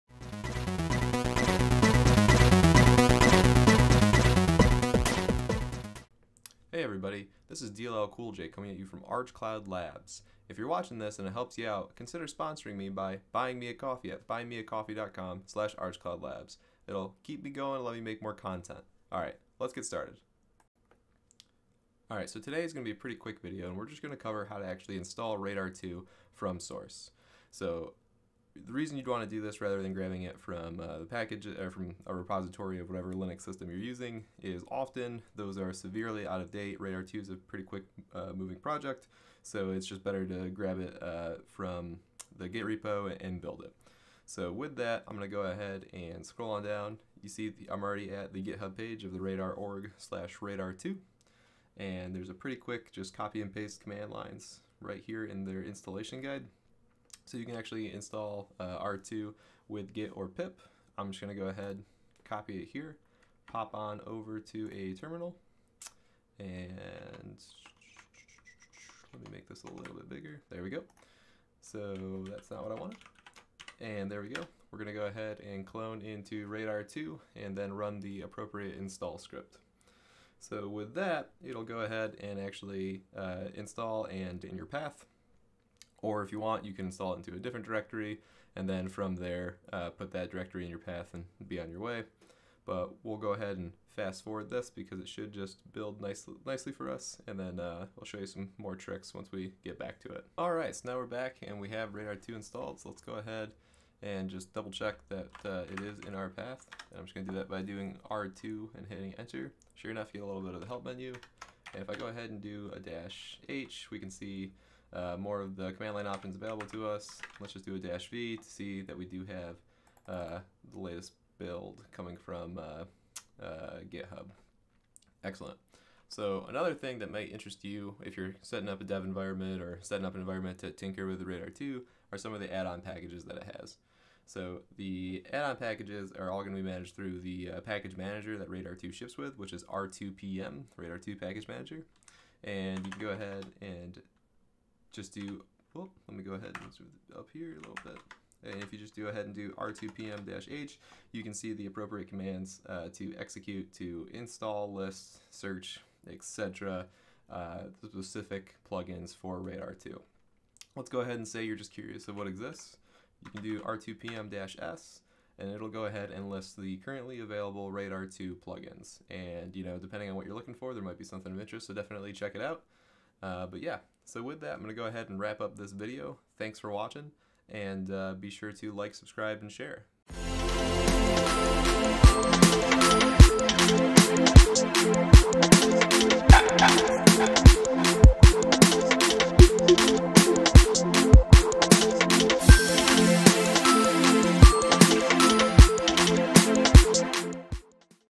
Installing Radare2
This video covers how to quickly install the latest radare2 version.